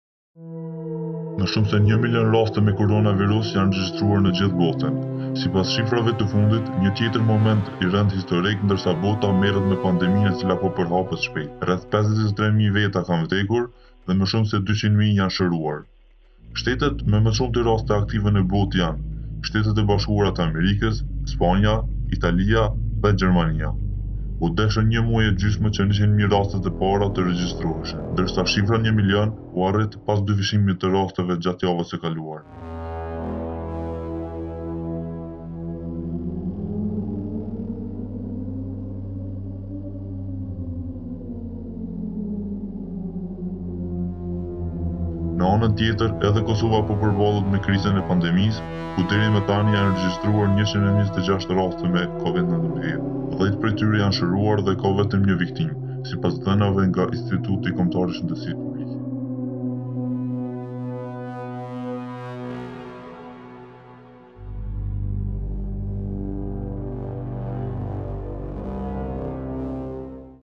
1 MILION raste me koronavirus në botë - 3 prill 2020
- 1 milion raste me Covid19 anë e mbanë globit
- 200 mijë persona të shëruar
- 50 mijë të vdekur

Gazeta TeleshkronjaPost